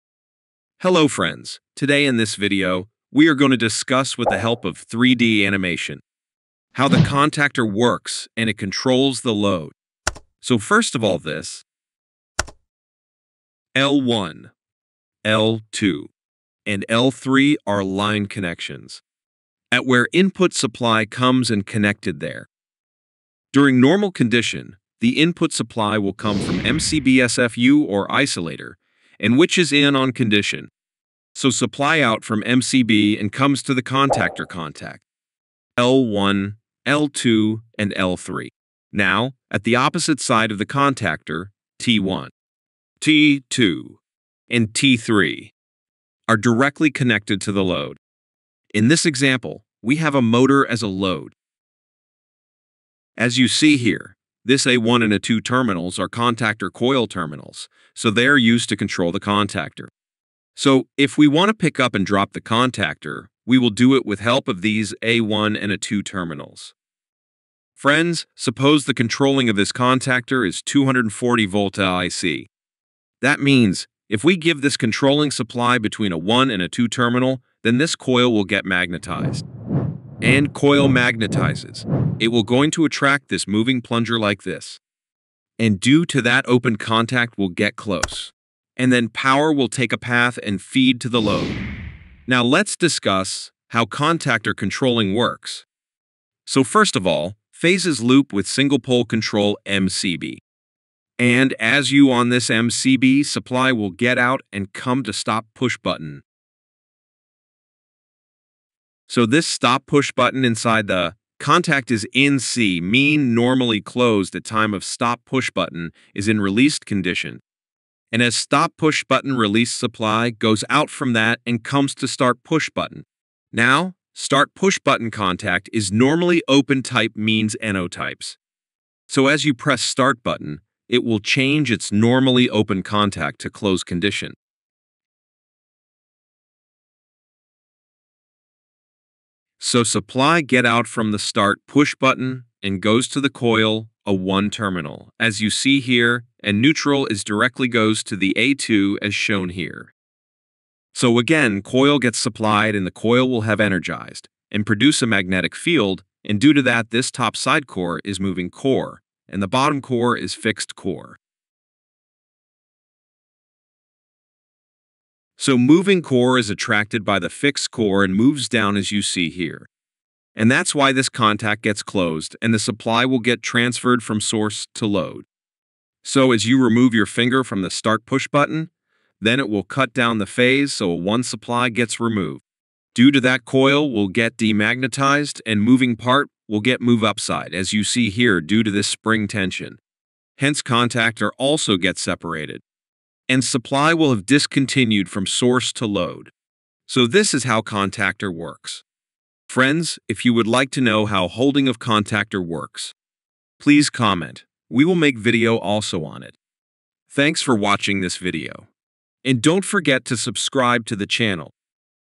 [ENGLISH] HOW CONTACTOR WORKS?
in this video CONTACTOR working, contactor function and why contactor used are explain